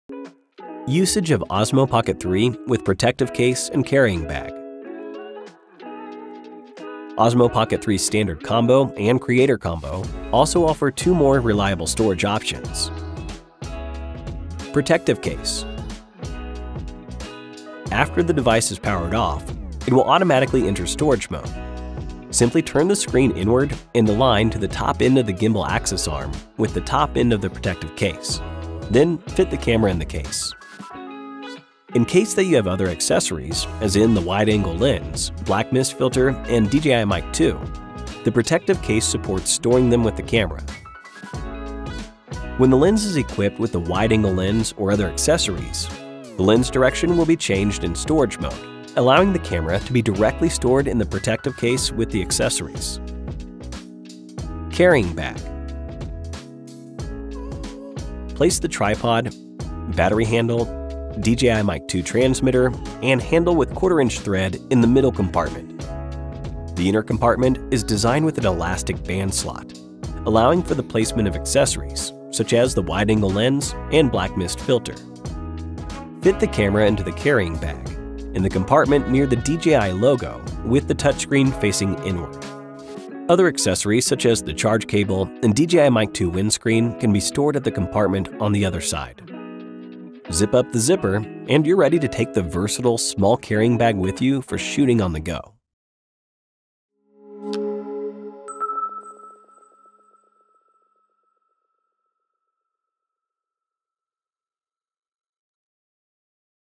Osmo Pocket 3｜Protective Case and Carrying Bag | DJI India
This video will assist you in using the DJI OSMO Pocket 3 for the first time.